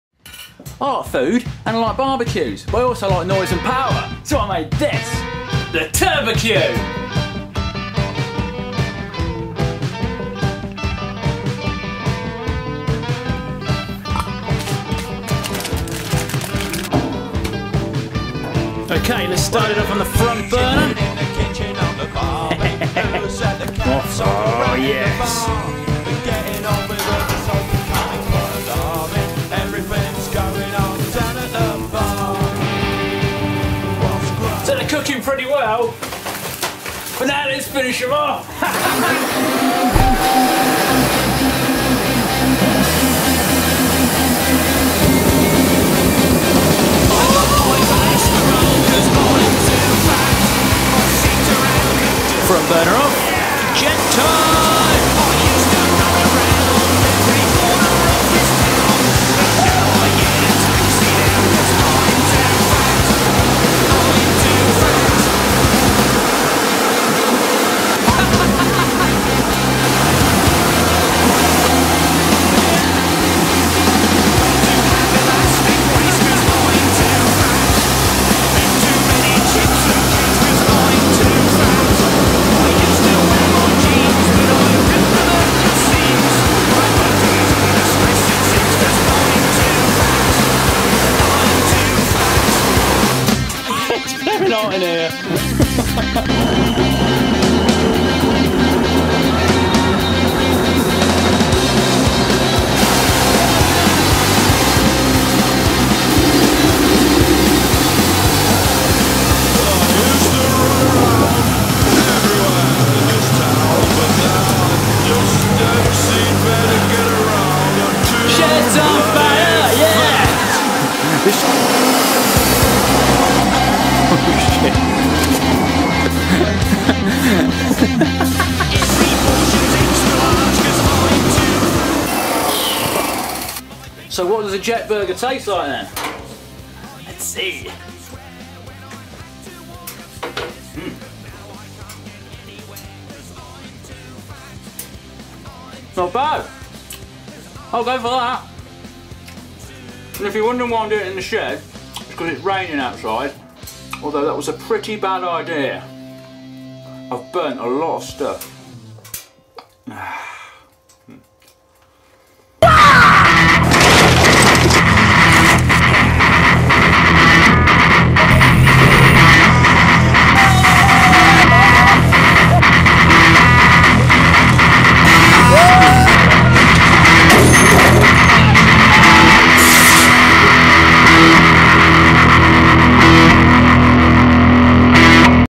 JET ENGINED BBQ.....The TURBAQUE
Hello Geek Week this is the TURBAQUE the only fully functioning BBQ in the world to have a jet engine for extra taste and brilliance.
Yes making this video in the shed was a terible idea but the british weather let me down and geek week was looming.

Music is by march to the grave as usaul and here is there itunes link